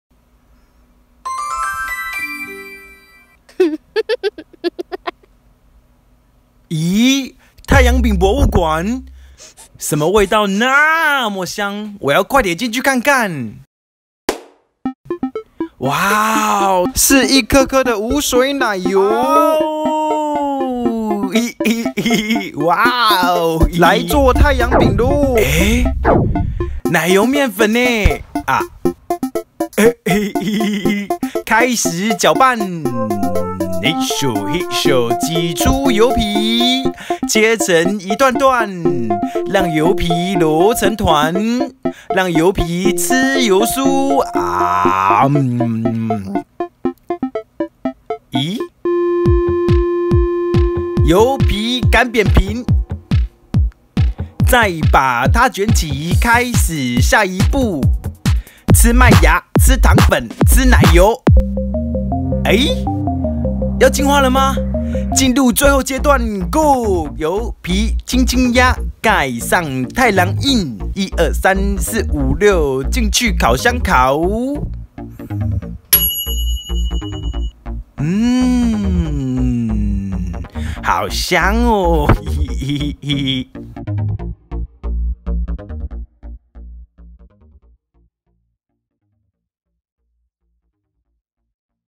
太陽餅博物館 一起去看看太陽餅夢工廠
你知道太陽餅是怎麼做的嗎？

和太郎一起去看看太陽餅夢工廠吧